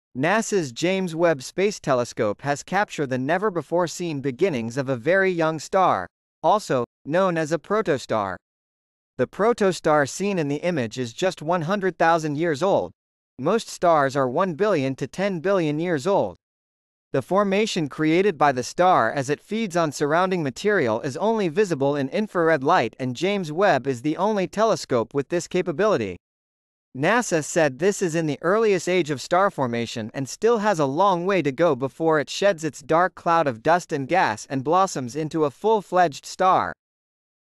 A star is born!
NASA's James Webb captures once-hidden beginnings of a very young star creating a fiery hourglass-shaped cloud as it feeds on surrounding matter to increase in size